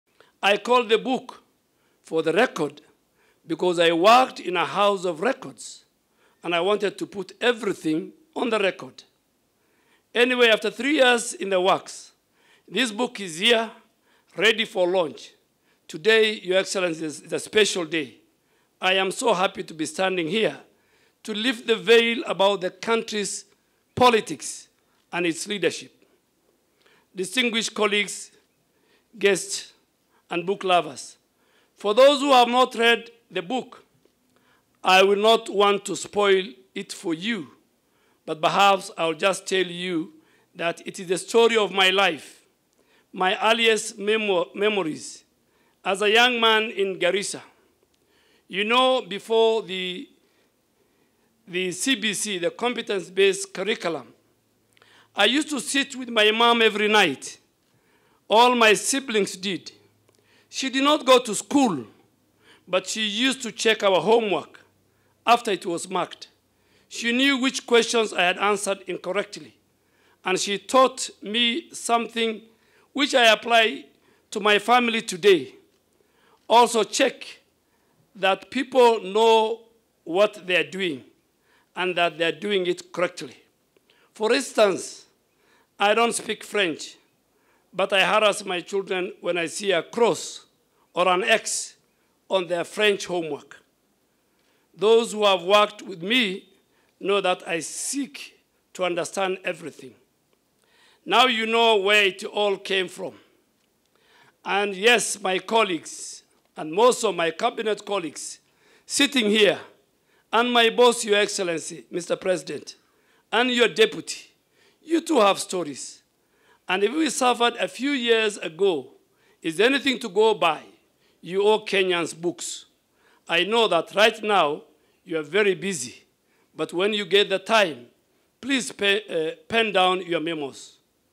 Aden Duale Book Launch, Why the title 'For the record'
President William Ruto at the Launch of The Record, A book on Politics and Betrayal by Aden Duale, Nairobi